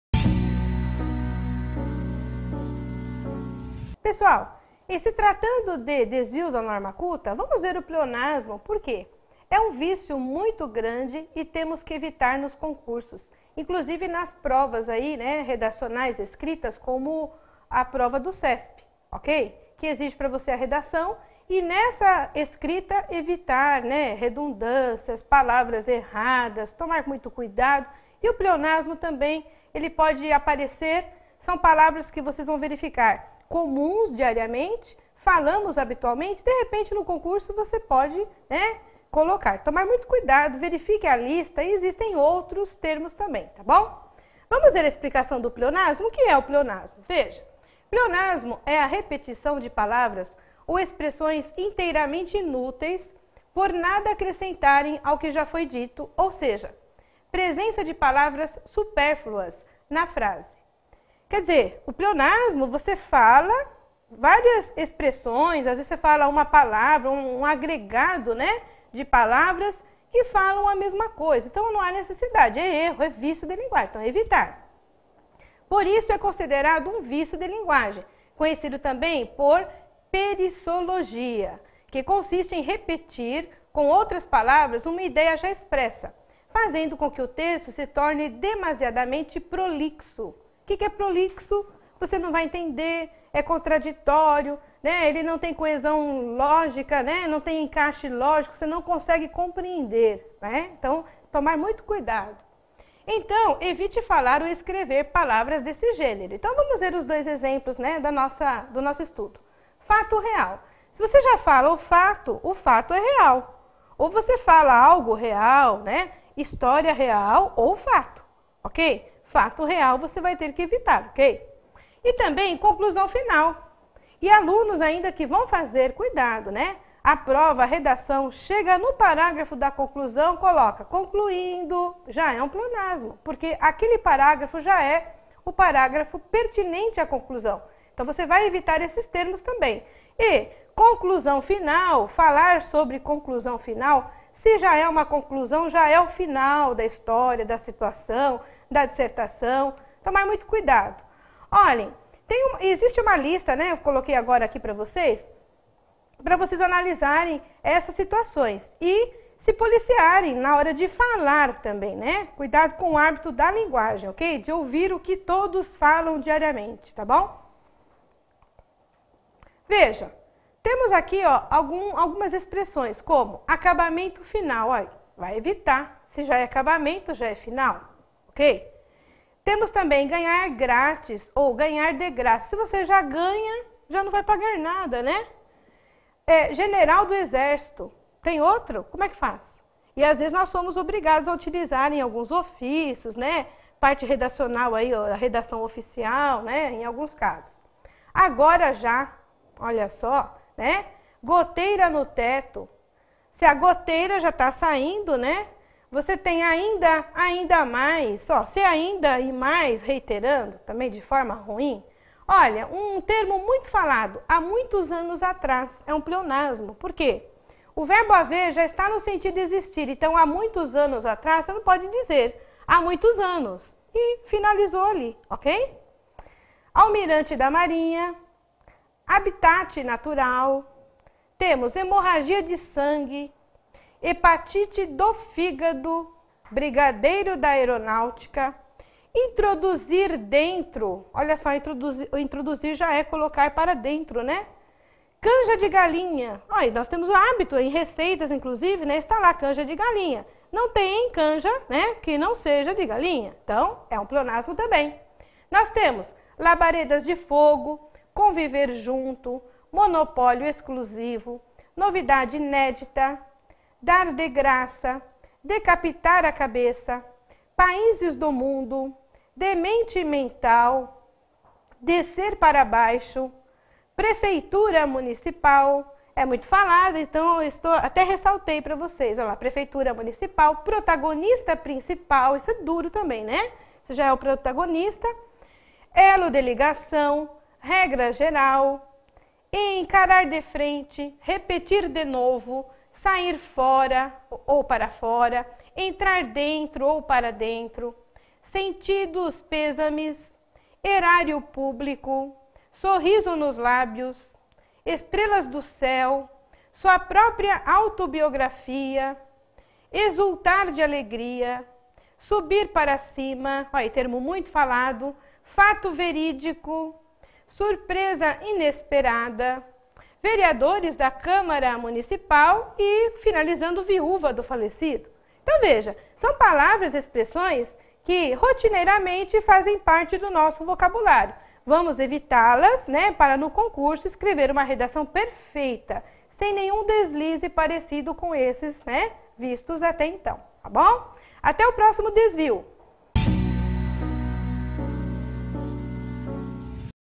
Aula Grátis - Português Interpretação de Textos - Pleonasmo - (videoaula 05)
Confira a videoaula (aula) grátis de Português Gramática (Aula 05) para quem estuda para concursos públicos e OAB.

CURSO DE PORTUGUÊS PARA CONCURSO GRÁTIS